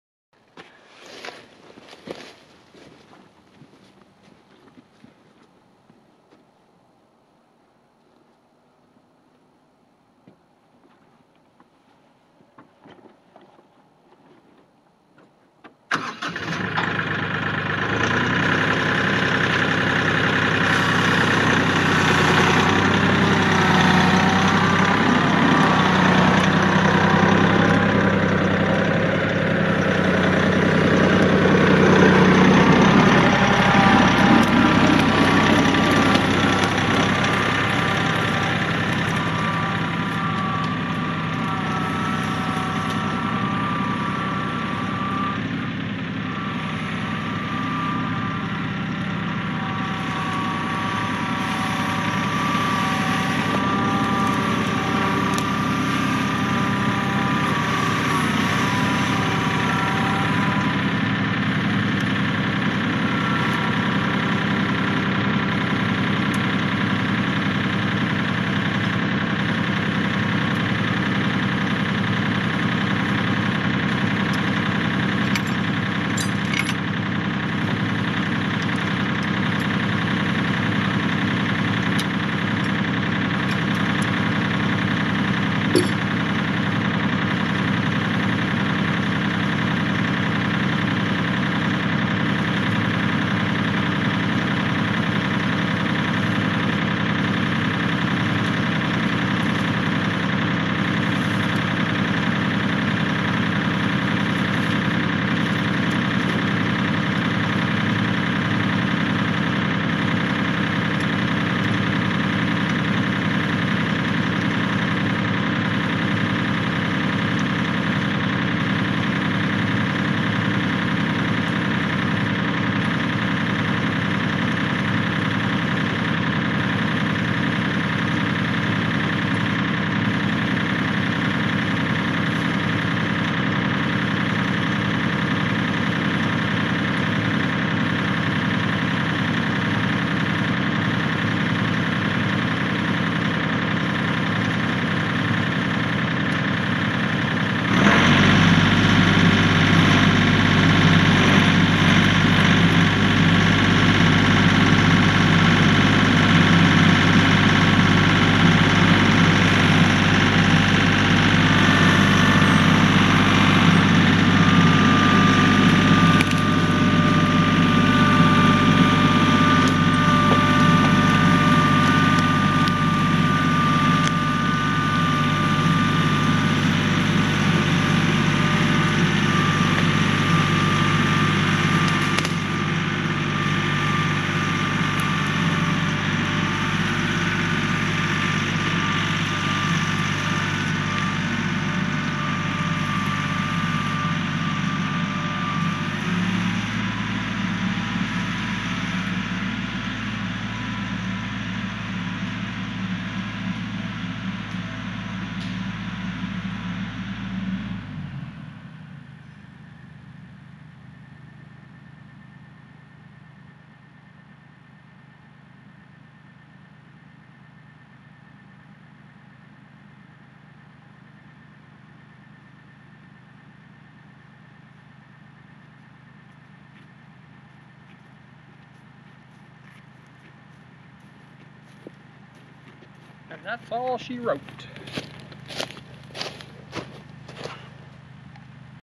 Tractor Logging Ash
Pulling a white ash log out of the woods with a Kioti 4010H tractor.